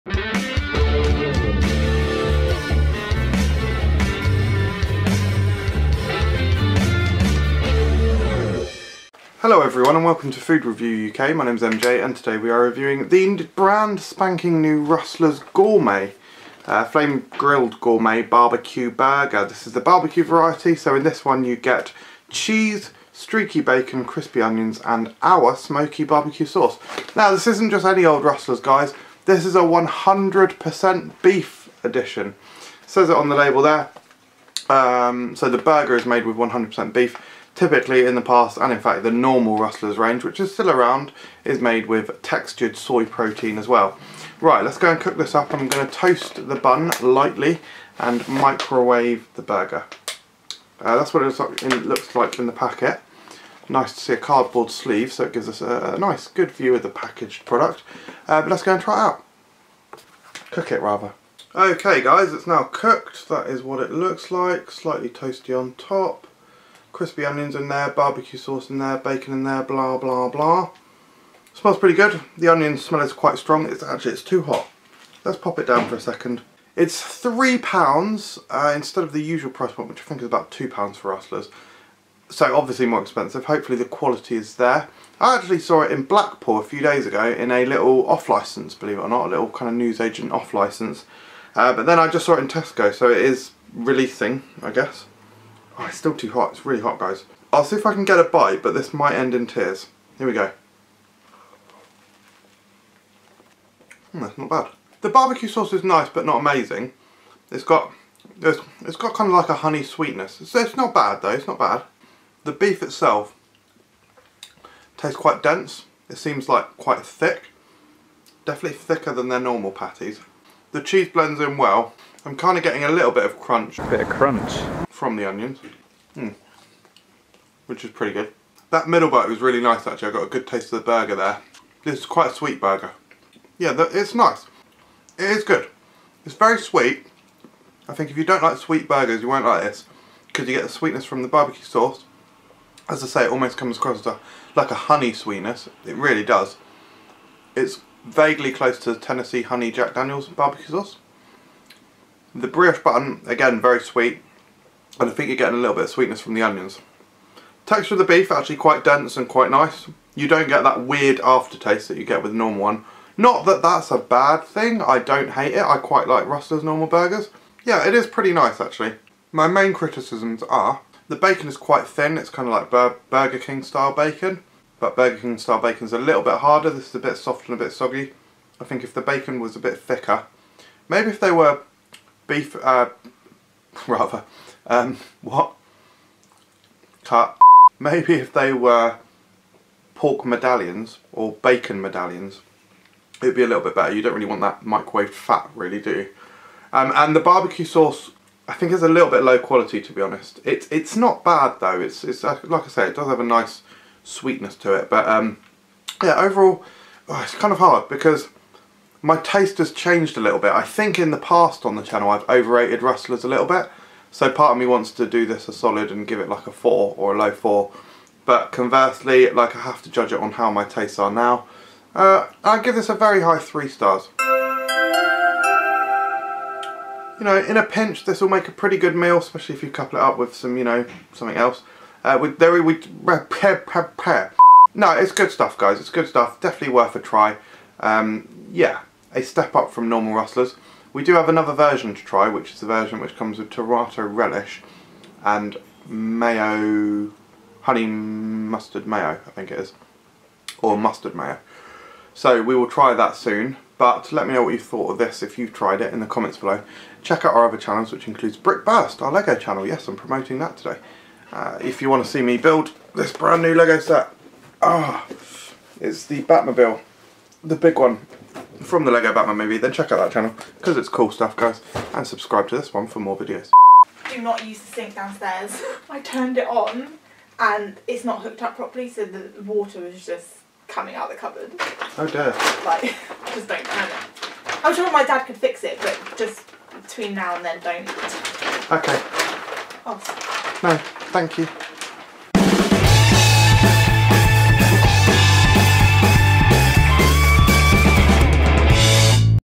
Rustlers Gourmet (100% Beef) BBQ Burger Review
MJ checks out Rustlers Gourmet burger range, this particular variety is the BBQ version with beef, bbq sauce, crispy onions, cheese and a brioche bun. SWEET STUFF.